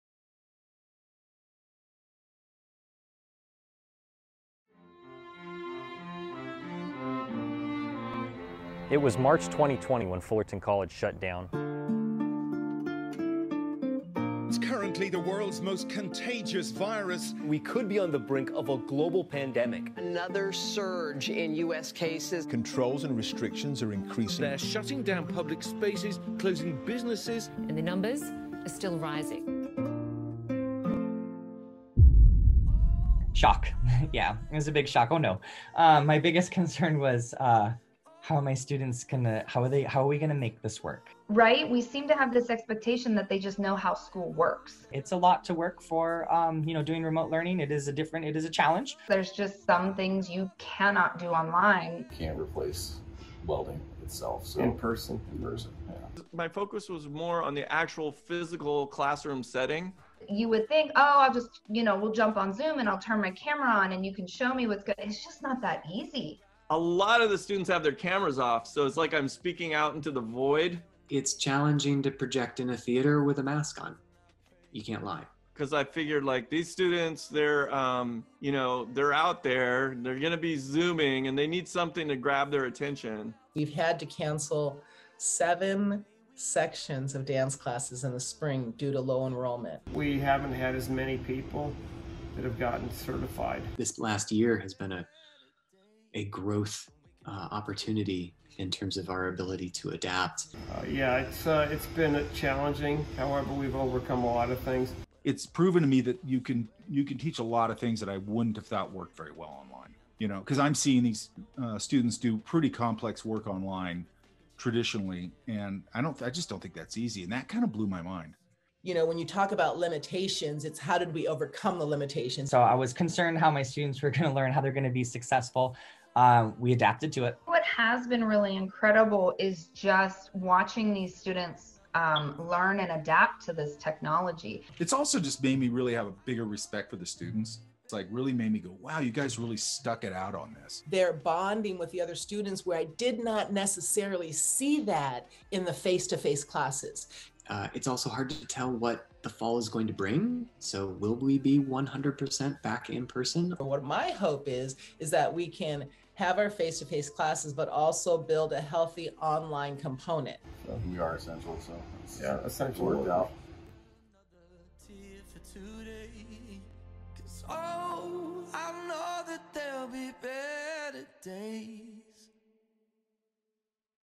Campus Update: Fullerton College One Year Later - Documentary Teaser (2021)
It was March 2020 when Fullerton College shut down, a year of trials and tribulations as students
and faculty adapted to virtual learning and the sights and sounds of students in this quad were no
more. A vibrant and busy place, now empty. Though with the battle of surviving the pandemic,
there are still stories of overcoming barriers and hope for the near future of a return to normalcy.

Documentary teaser by Myron Caringal.

Drone footage courtesy of Jay Seidel.